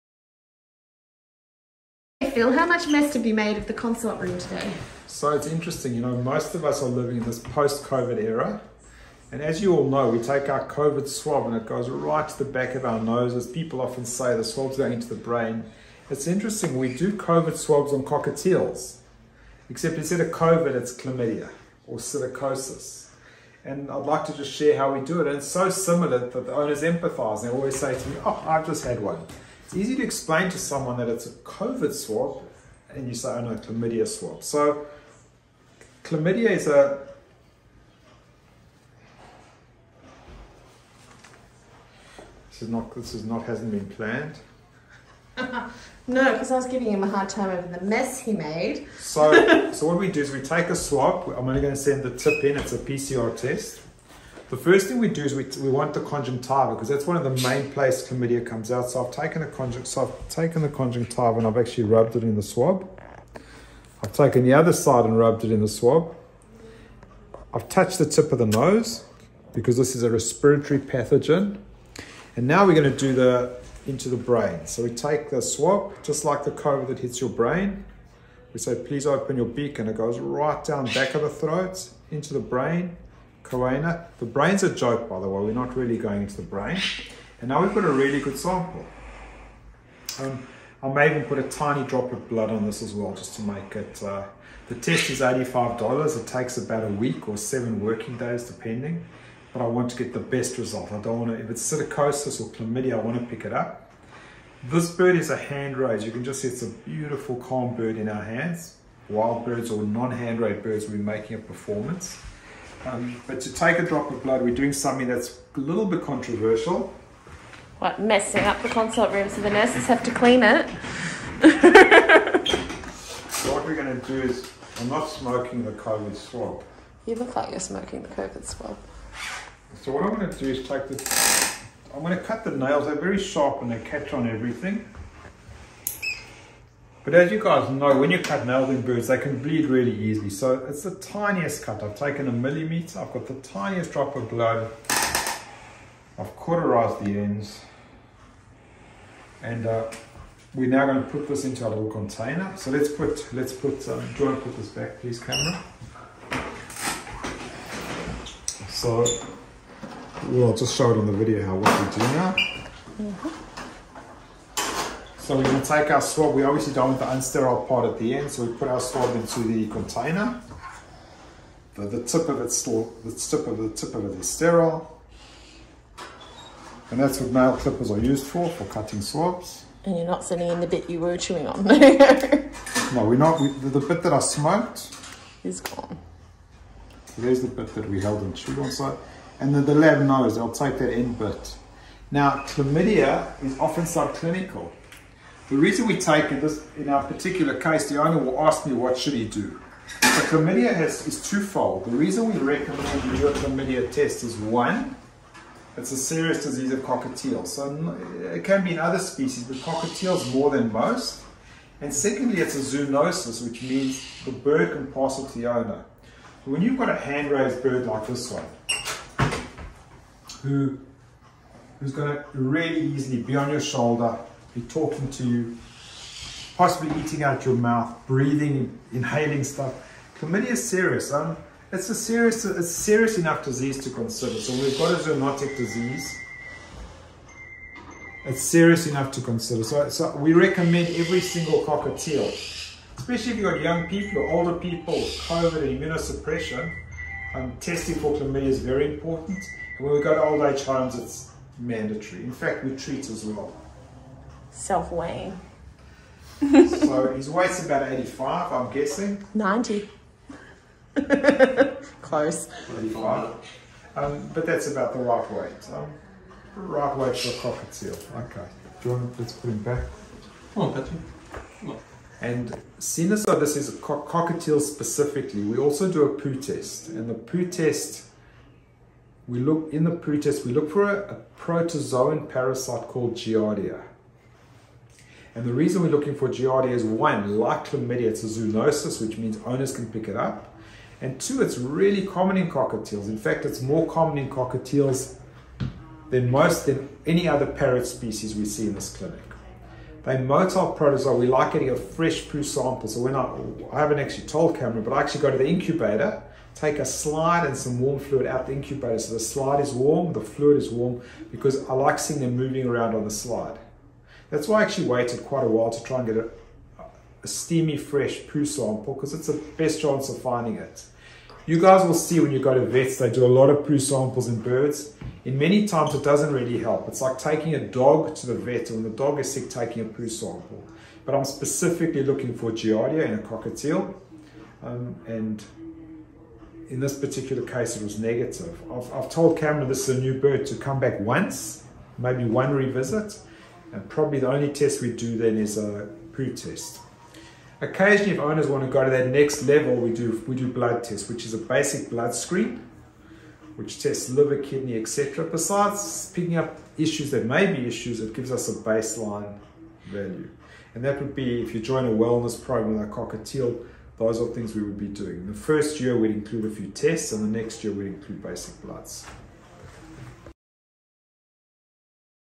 Testing for Chlamydia (Psittacosis) in parrots
Psittacosis (commonly called chlamydia) is often found in certain types of pet birds in Australia.  Cockatiels are one of the breeds that are most commonly affected.
It is highly contagious and it's zoonotic which means that humans can catch this from their pet bird.
Common symptoms are flu like in both humans and birds - in fact it's not unlike covid-19.
Fortunately, it is very easy to do a screening test and if found it is usually relatively easy to treat.
In this video,  Dr Phil demonstrates how to take a sample for a PCR screening test.  (There are other tests such as an immunocomb, which are not shown here.)
It should be noted that this is a worthwhile screening test to do if you are adding a new bird to your flock in Australia.  In fact, the more responsible bird boarding facilities require this screening test be done before they will board your bird.